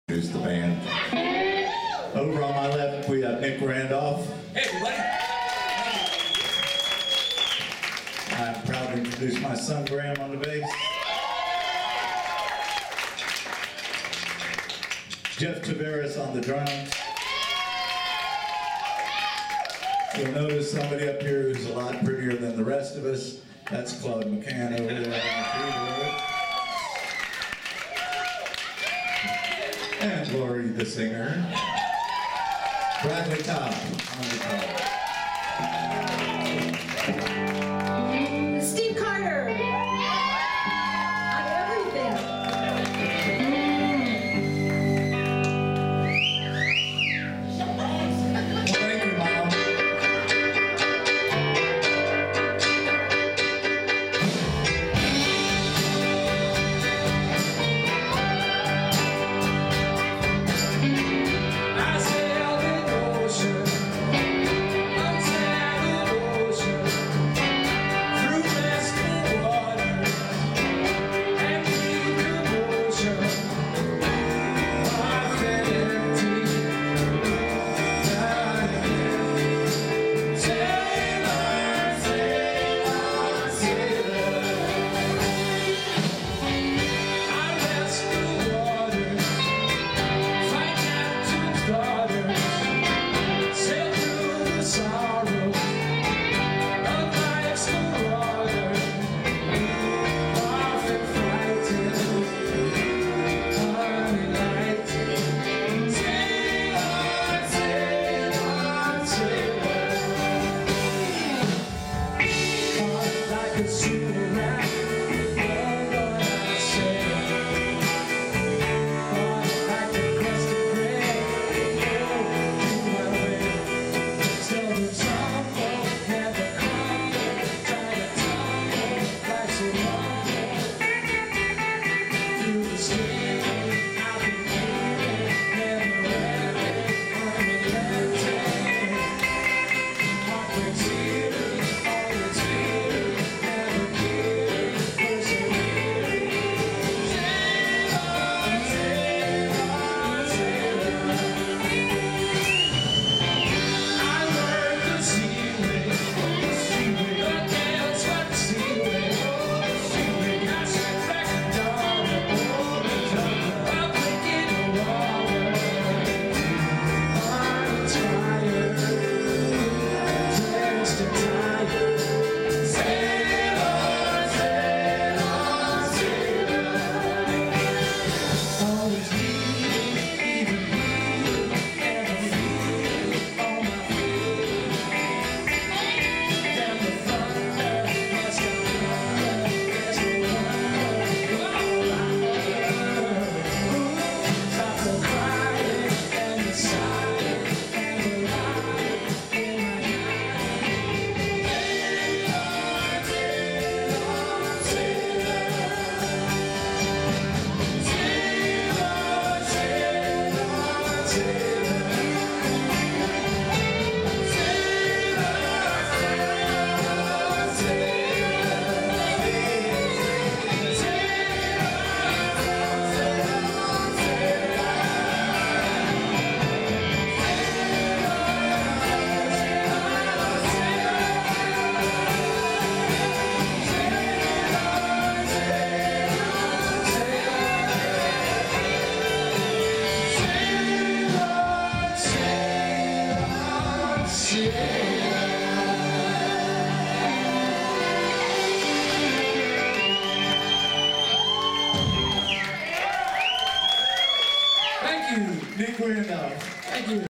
One 2 One bar from the Beach Boys Songbook
with Nick Randolph-lead vocals and Lorrie Singer, Claude McCan, Steve Carter, Graham Carter, Bradley Kopp and Jeffery Tveraas